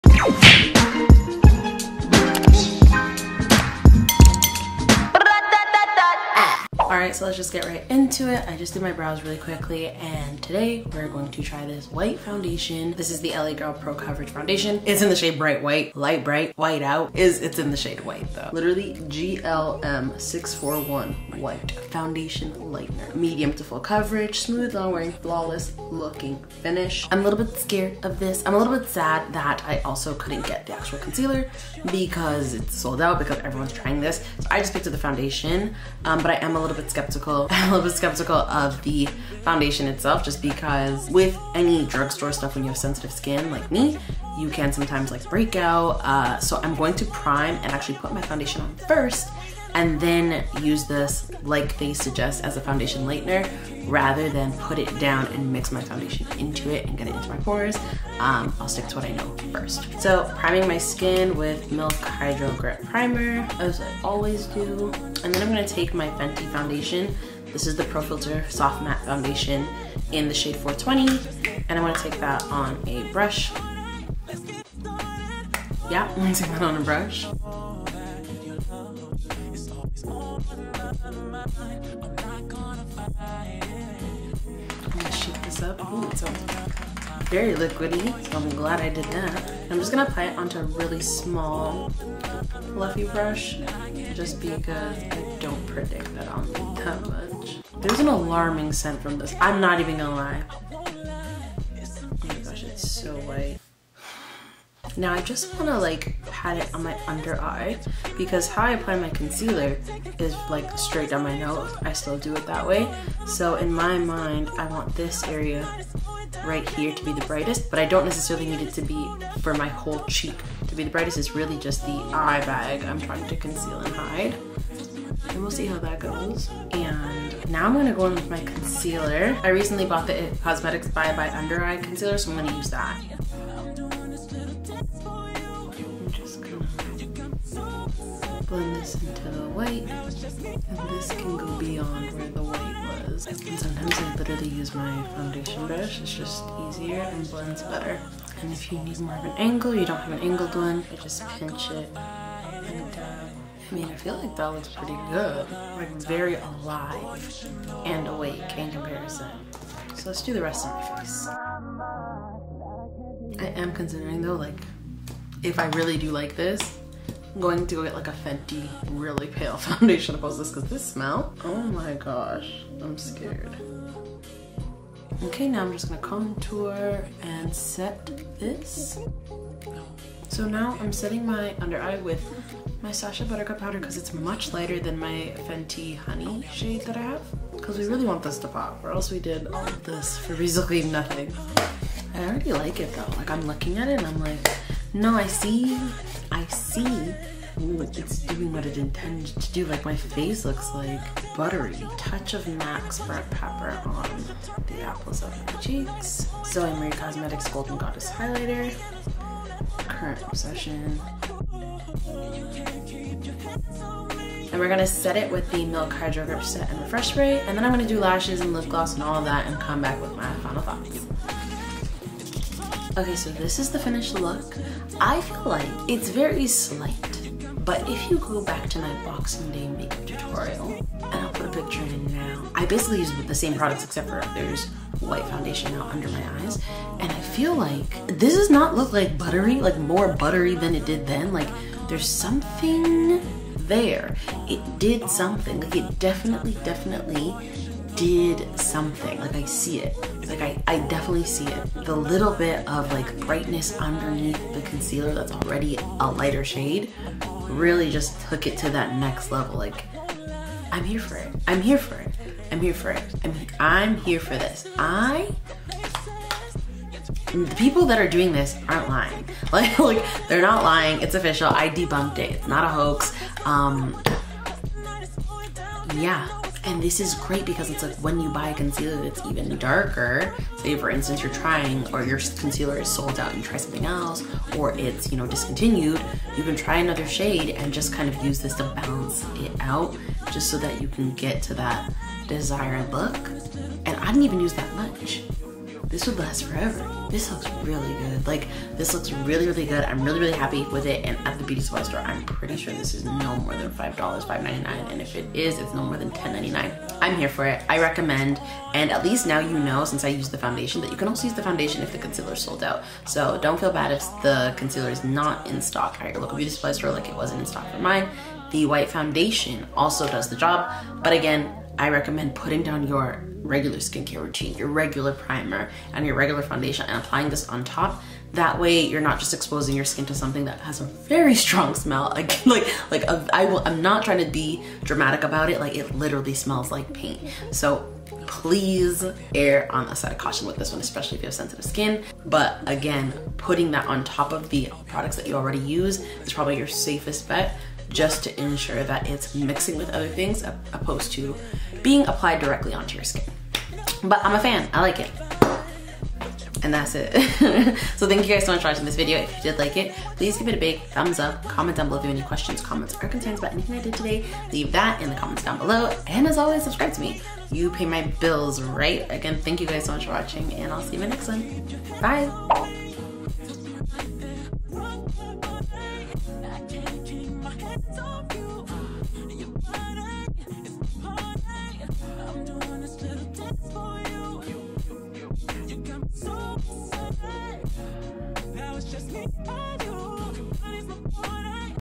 I tried the white foundation hack… ⎮ Natt Gee
FAQ:
Age - 28 ☘️
Ethnicity - Jamaican 🇯🇲
Location - Canada 📍

SWEATINGO CODE: NATTGEE8
Your Radiance Spin-Care System: $28.50

PRODUCTS USED --
MILK MAKEUP Hydro Grip Primer
FENTY BEAUTY Pro Filt’r Soft Matte Liquid Foundation in 420
LA Girl Pro Foundation in White
IT Cosmetics Bye Bye Under Eye Full Coverage Concealer in Rich Golden (to highlight) & Deep Golden (to contour)
Fenty Beauty Sun Stalk'r Instant Warmth Bronzer in Mocha Mami
MAC Cosmetics Burnt Pepper blush
Zoe Marie Cosmetics Golden Goddess Glow highlighter
MILK MAKEUP Hydro Grip Setting + Refreshing Spray
The Luxe Beautipolitan Charming - Siberian Mink Lashes USE COSE NATGEE TO SAVE $$$
MAC Cosmetics Lip Pencil in Chestnut
Zoe Marie Cosmetics Lustre Lipgloss

TAGS --
white foundation, white concealer, foundation lightener, makeup hacks,